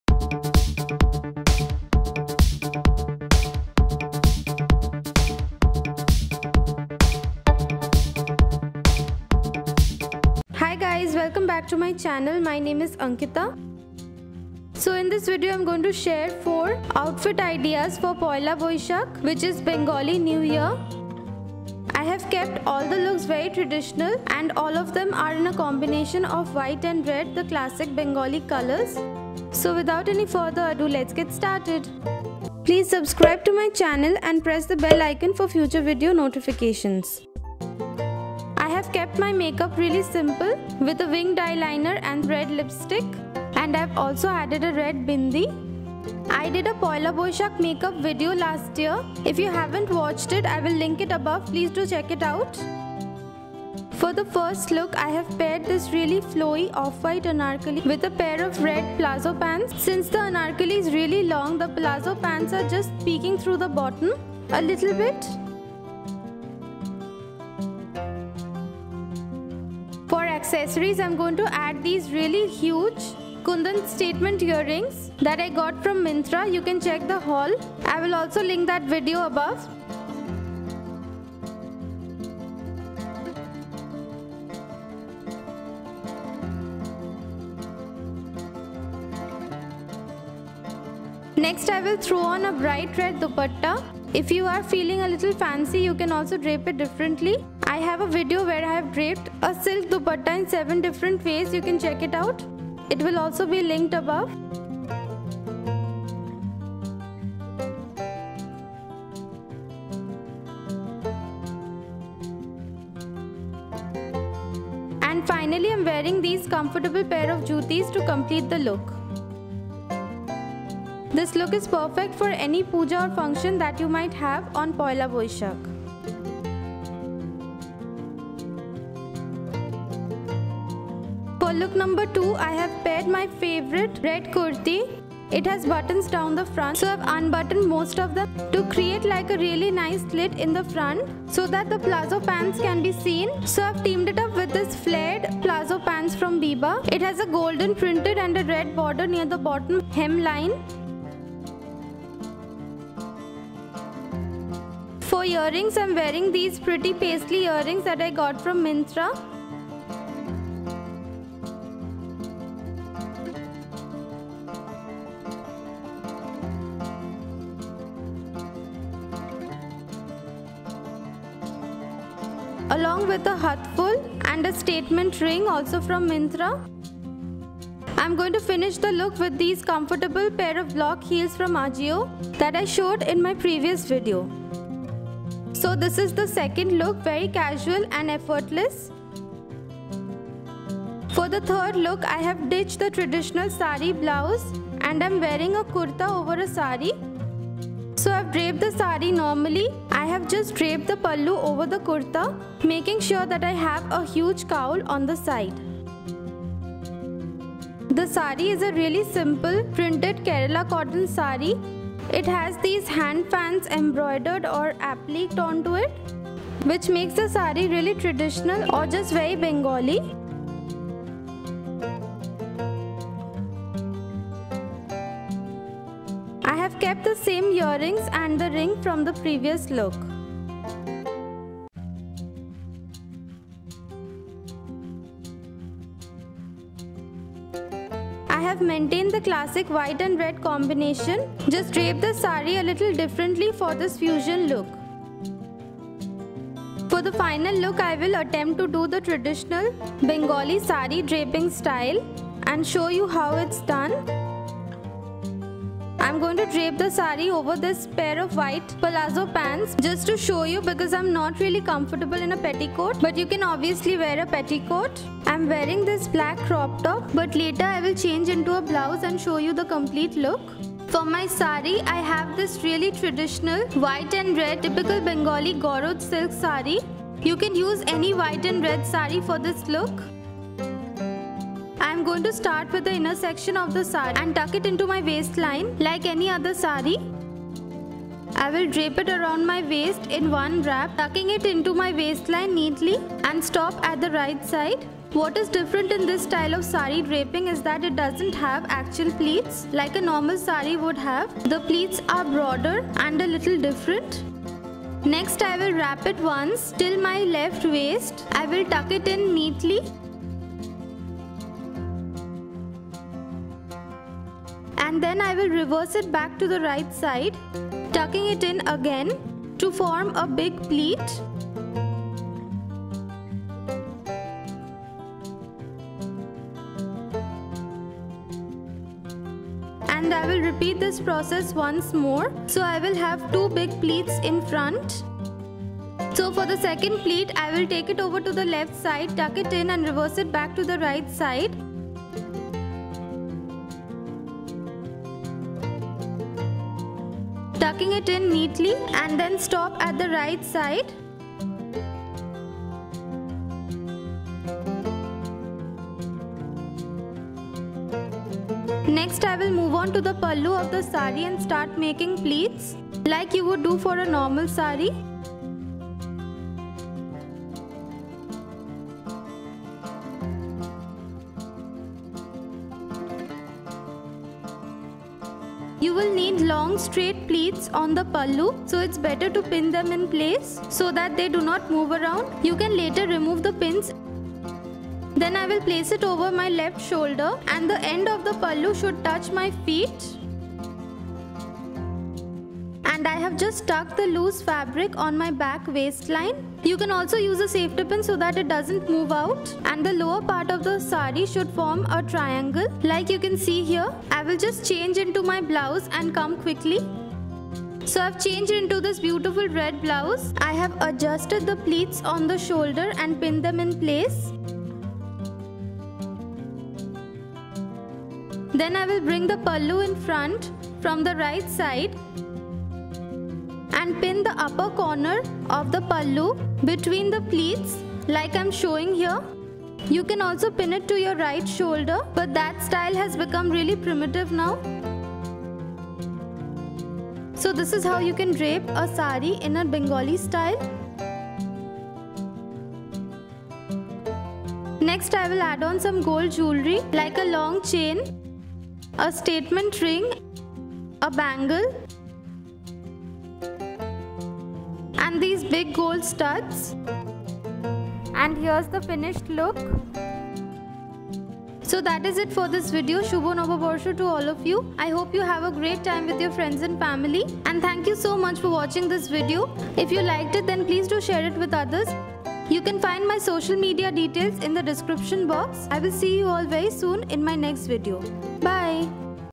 How to Drape a Saree in Bengali Style | Red & White Outfits | Poila Boisakh Outfit Ideas
In this video, I will share with you 4 different Red & White Outfit Ideas for Poila Baisakh. I'll also show you how to drape a saree in bengali style.

Look 1:

- Kurta: Aks
- Palazzo Pants: Pantaloons
- Dupatta: Hatibagan, Kolkata
- Earrings: Myntra
- Shoes: Ajio

Look 2:

- Kurta: Ajio
- Palazzo Pants: Biba
- Hathphool: Barabazar, Kolkata
- Earrings: Myntra
- Ring: Myntra
- Shoes: Ajio

Look 3:

- Kurta: Global Desi
- Saree: VIP Market, Kolkata
- Earrings: Myntra
- Ring: Myntra
- Shoes: Ajio

Look 4:

- Saree: Kolkata
- Blouse: Stitched
- Gold Jewellery
- Shoes: Ajio

Equipment used:
Camera: Canon EOS 700D ( Click to buy
MIC: Rode VideoMicro Compact On-Camera Microphone ( Click to buy
Tripod: Similar one ( Click to buy